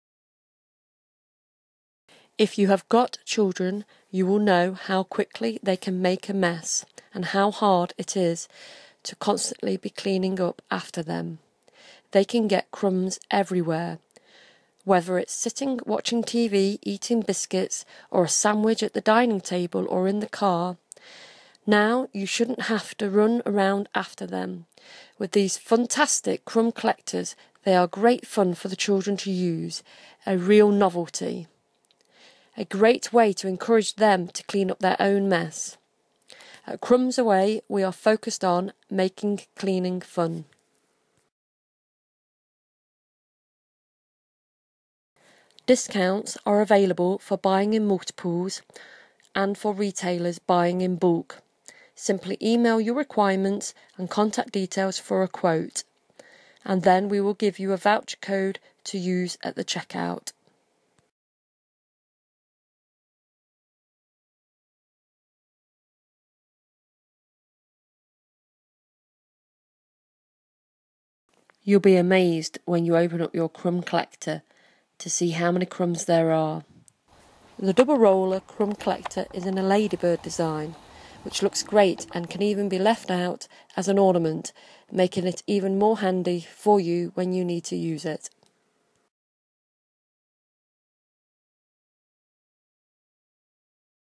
crumbsaway.co.uk DISCOUNT at end of video - FUNtastic crumb collector carpet sweeper brush
Carpet sweeper, crumb collector brush.  Sweep away crumbs in seconds with your carpet sweeper. These FUNtastic carpet sweepers are easy to use on your carpets, rugs, sofas, chairs and handy to have for the home, car, caravan, boat.  Roll the crumb brush side to side and watch the crumbs disappear. These crumb collectors are a novelty for children and a great way to encourage them to clean up their own mess.